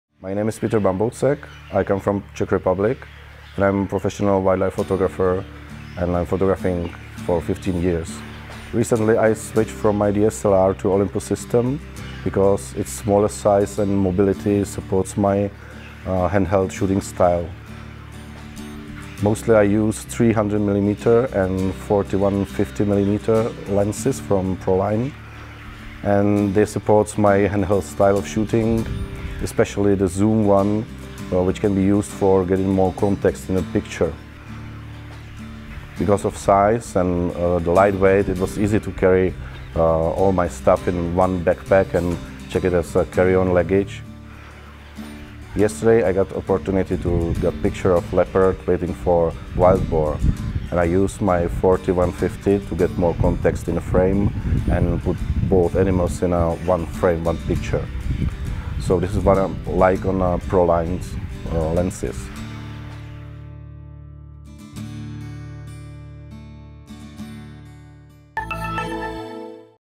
Behind The Shot with PETR BAMBOUSEK (Spanish)
Petr Bambousek is a professional photographer who specialized in wildlife shots. For these kinds of shots he often uses the M.Zuiko Digital ED 300mm F4.0 IS PRO and the ED 40-150mm F2.8 PRO lens.